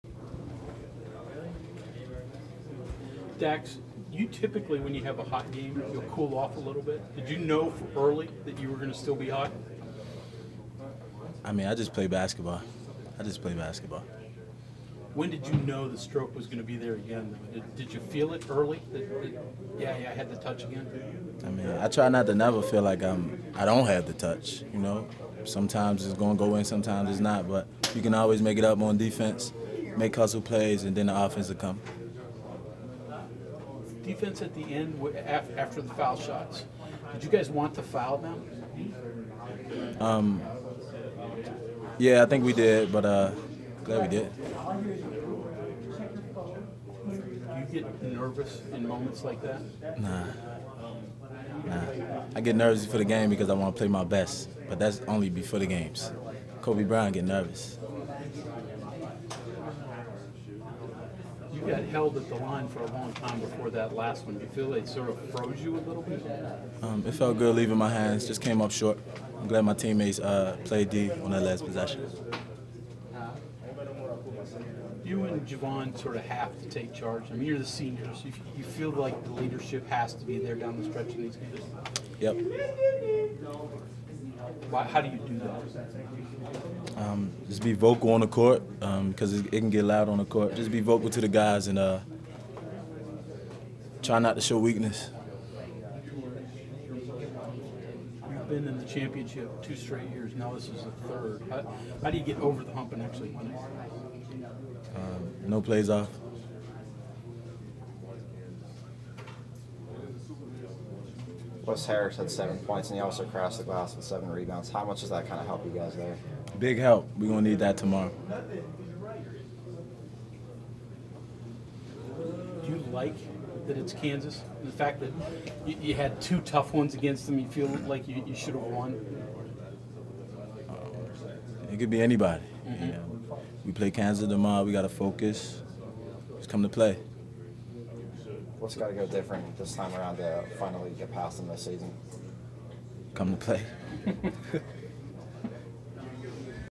BlueGoldNews.com: Daxter Miles WVU - Texas Tech Postgame 030918
West Virginia guard Daxter Miles discusses his 22-point effort in the Mountaineers' 66-63 win over Texas Tech in the semifinals of the Big 12 Championship